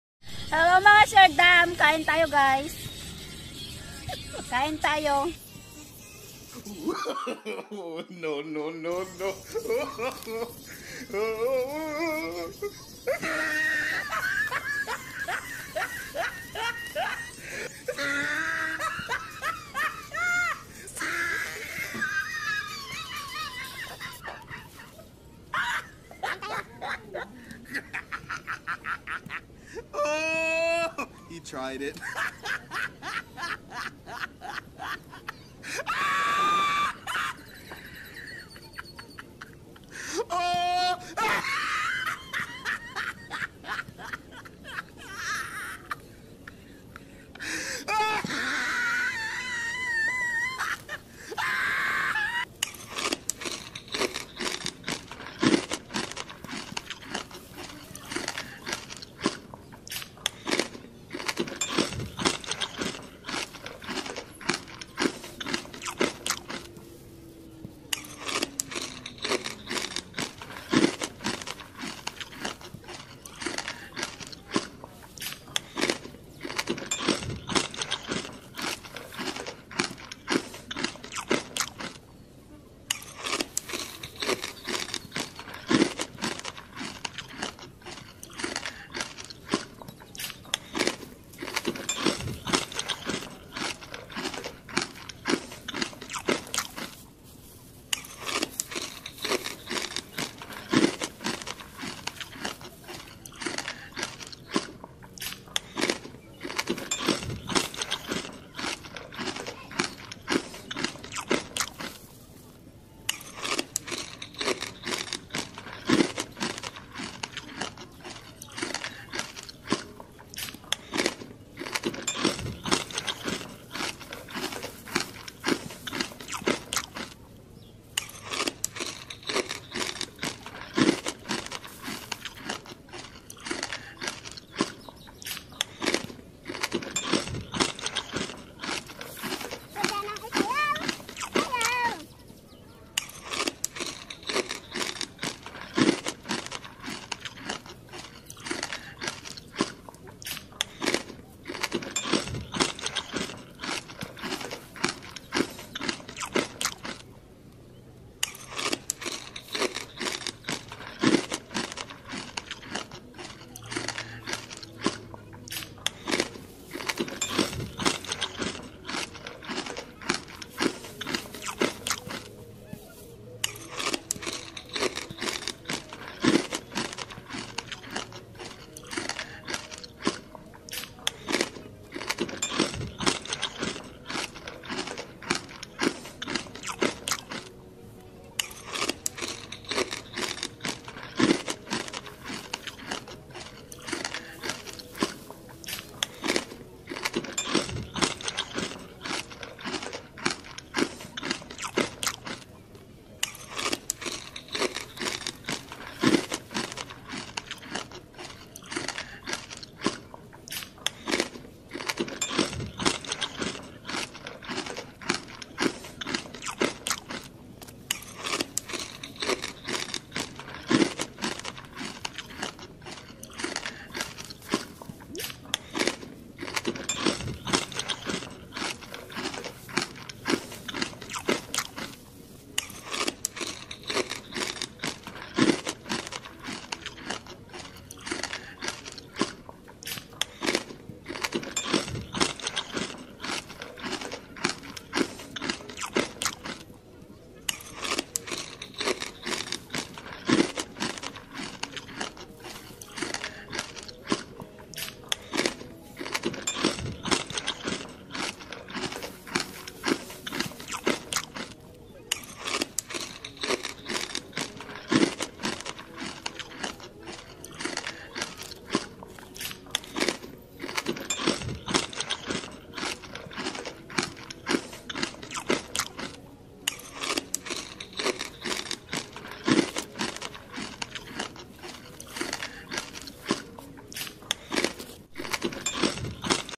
NAGUTUM .PARANG ISANG TAON D KUMAIN HAHA
Isang crispy pata inubus ko haha nagutum ako kakalakad .grabe busug na busug ako haha